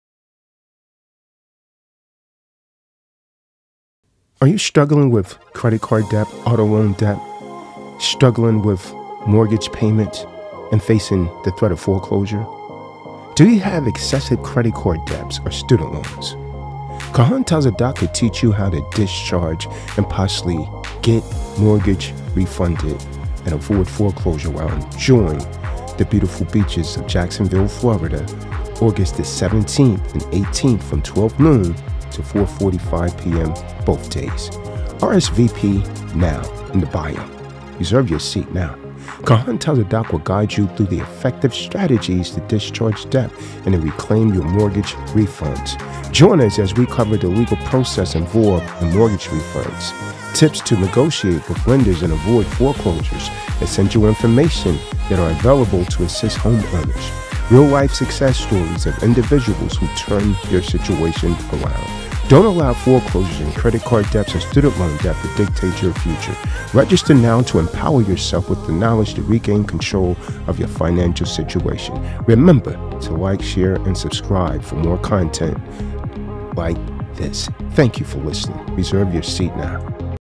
Discharge Debts, Save Your Home From Foreclosure Get a Mortgage Refund
Are you facing foreclosure and feeling overwhelmed by debt? In this informative video, we’ll guide you through effective strategies to STOP foreclosure and secure a mortgage refund FAST! Discover practical steps to regain control of your financial situation, and learn how to negotiate with lenders, understand your rights, and explore options available to help you avoid losing your home. Whether you’re struggling with payments or looking for ways to reclaim your hard-earned money, this video is packed with valuable insights that can make a difference. Don’t wait until it’s too late—watch now and take the first step towards financial freedom!

Don't let foreclosure dictate your future. Register now to empower yourself with the knowledge to regain control of your financial situation!
Discharge Debt, stop foreclosure, and get a refund on your mortgage. Reserve your seat now